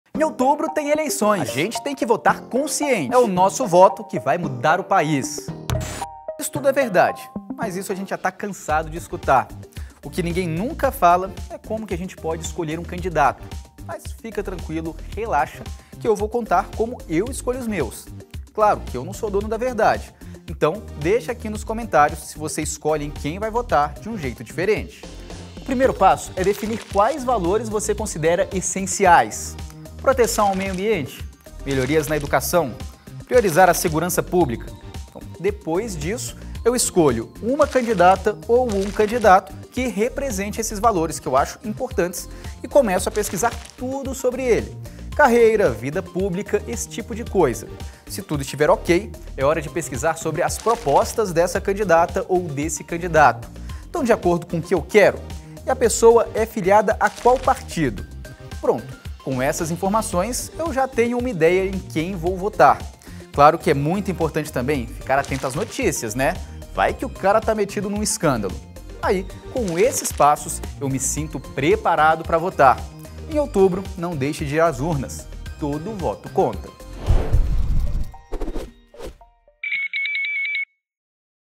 Como escolher um candidato
Neste ano teremos uma das principais eleições da história. E a escolha dos candidatos está nas mãos das eleitoras e eleitores. Na hora de votar, é importante pesquisar a fundo o histórico das candidatas e candidatos que você deseja eleger. Também é importante conhecer a fundo as propostas e ficar atento se os valores são compatíveis com o você acredita. O voto consciente ajuda a fortalecer a democracia. A eleição é feita por todos. Todo voto importa.

No site do TSE, você encontra respostas para todas as dúvidas que tiver antes de votar. Acesse

Ficha Técnica:
Reportagem: Henrique Amaral
Edição: Joel Lucas e Roberta Boni